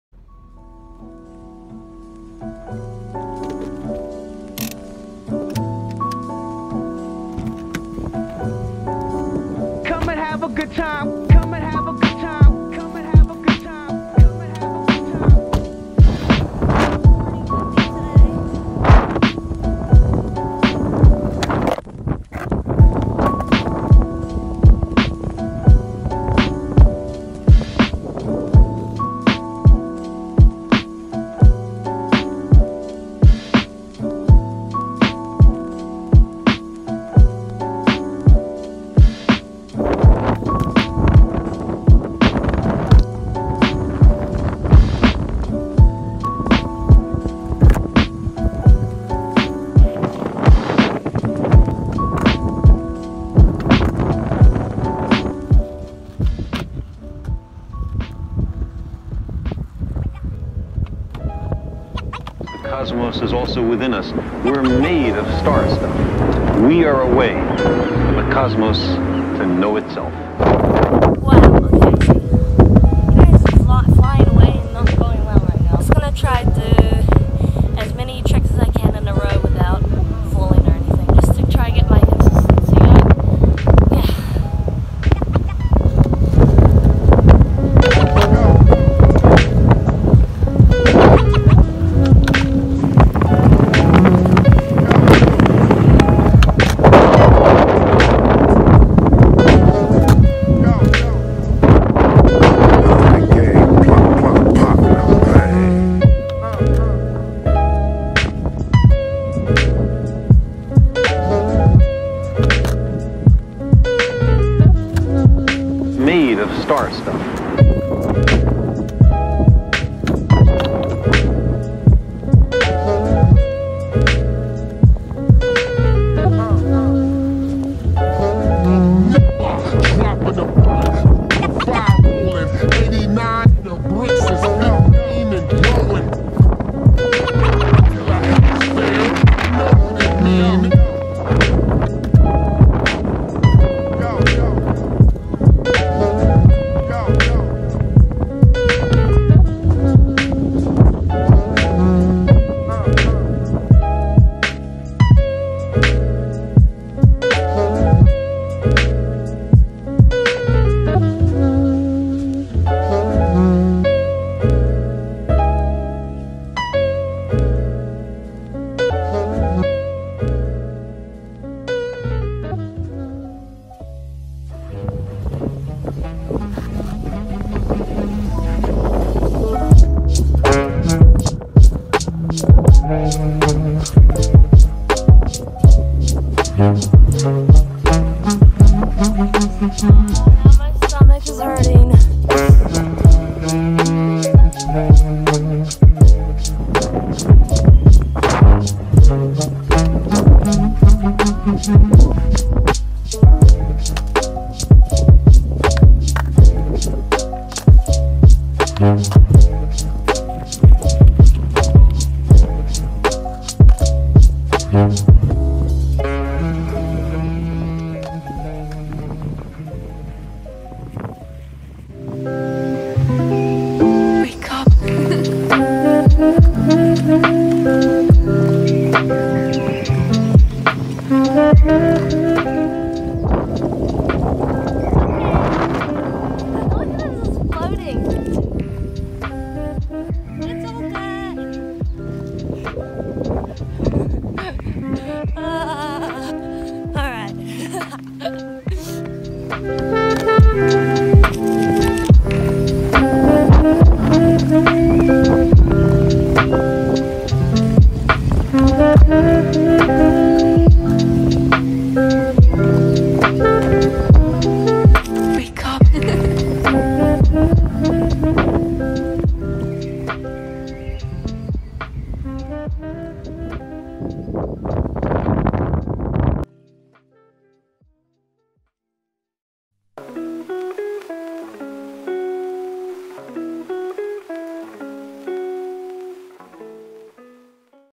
DITCH DAY | WINDY SKATE VLOG
Just a lil flat ground practice